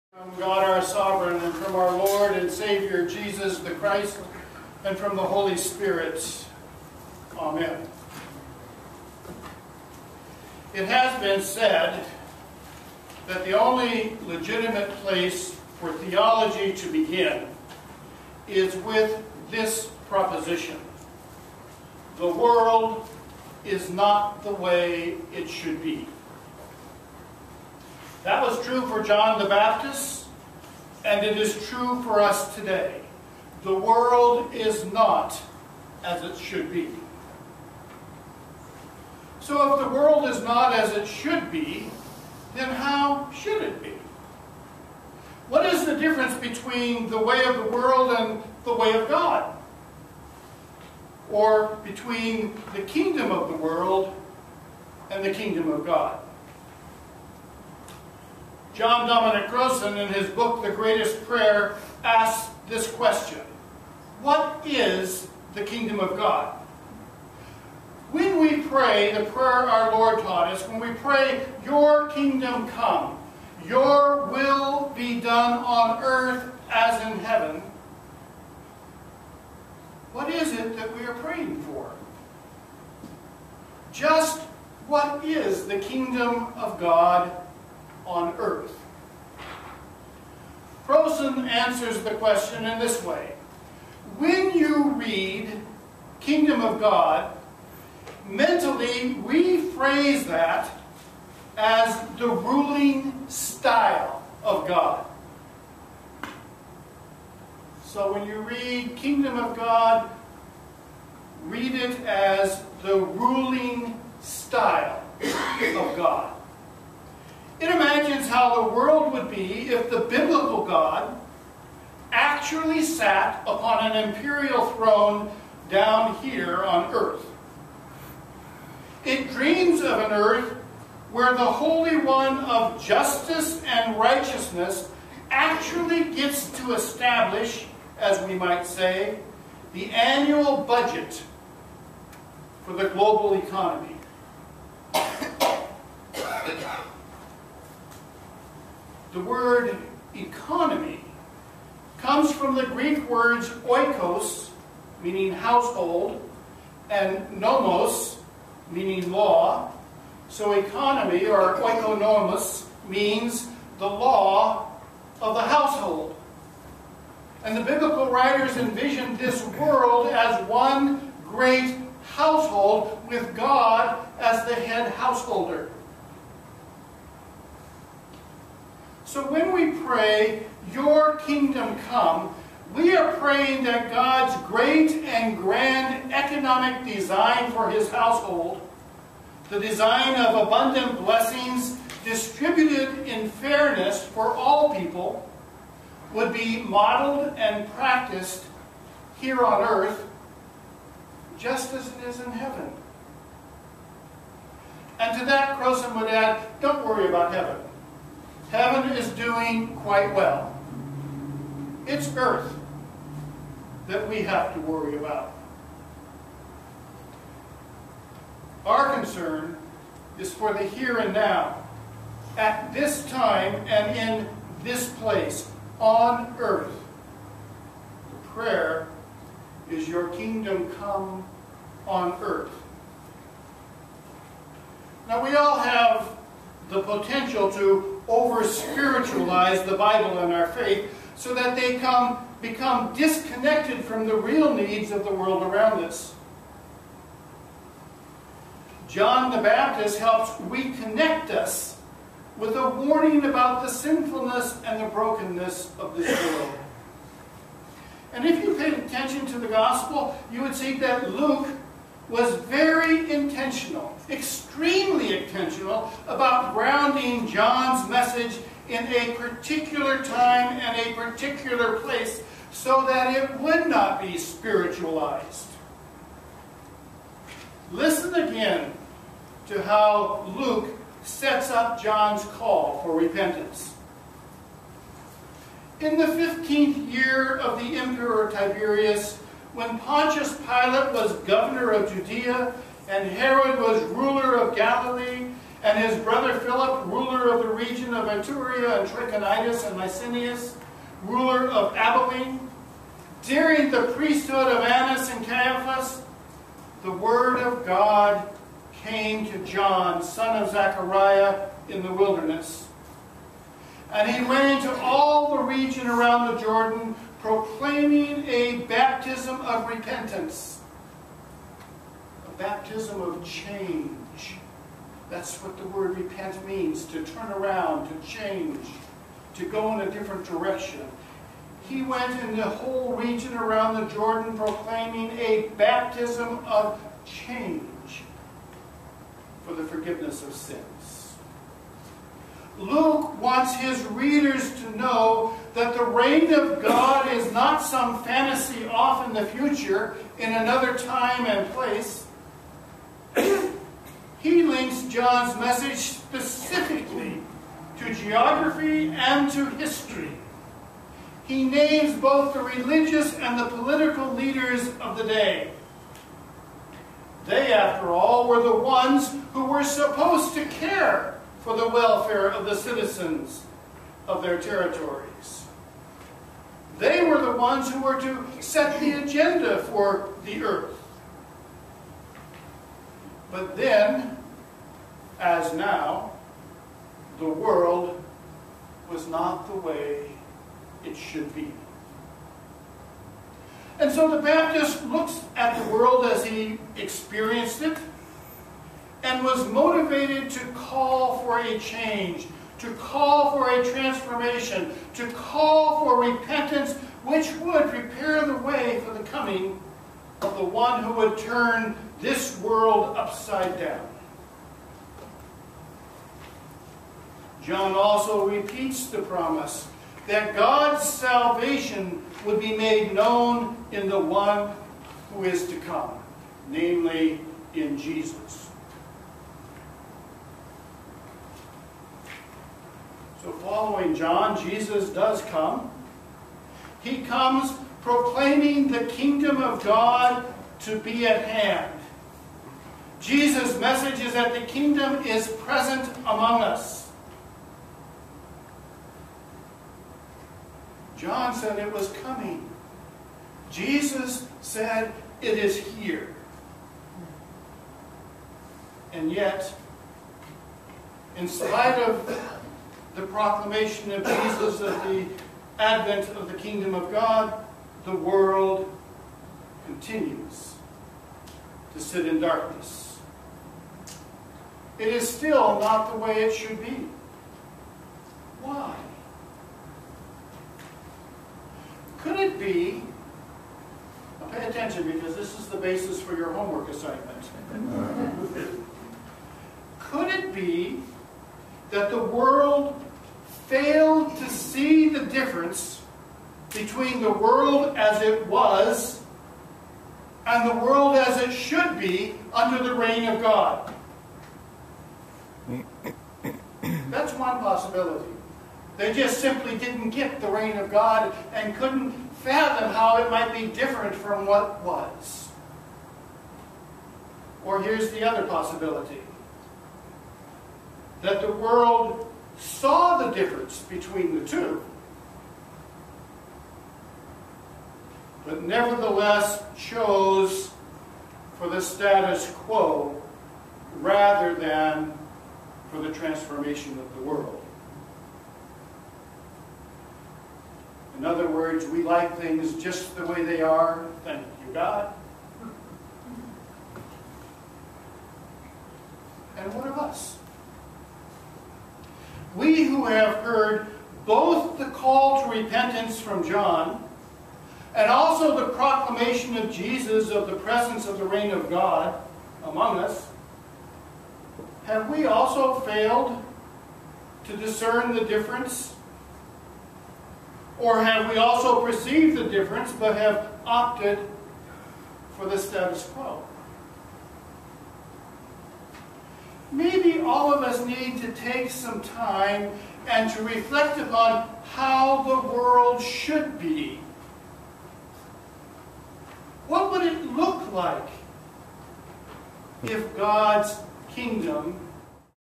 Pastor Vernon on the Kingdom of God on earth first half
Pastor Vernon preaches his second sermon at Gethsemane